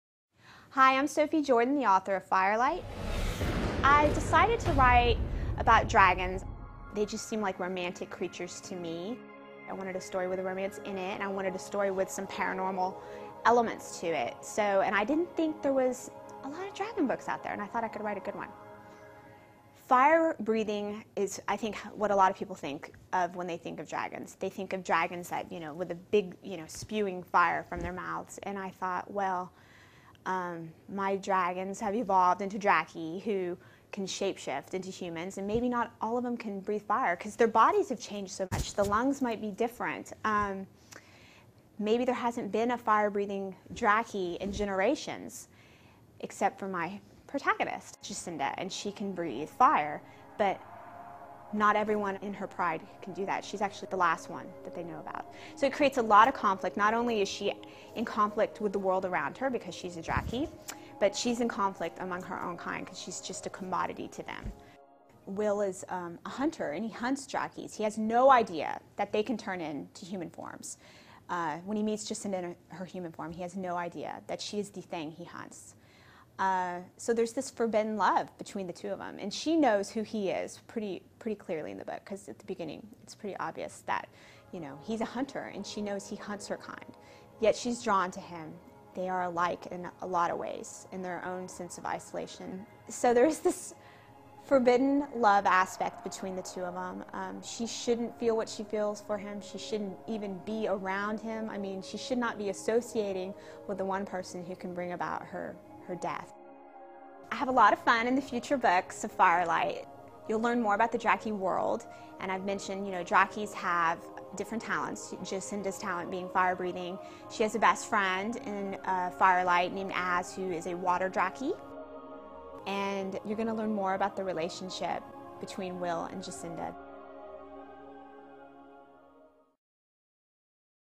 Entrevista a Sophie Jordan: autora de Firelight
La autora cuenta cómo imaginó esta historia y los secretos de los draki y Jacinda, la protagonista de la serie.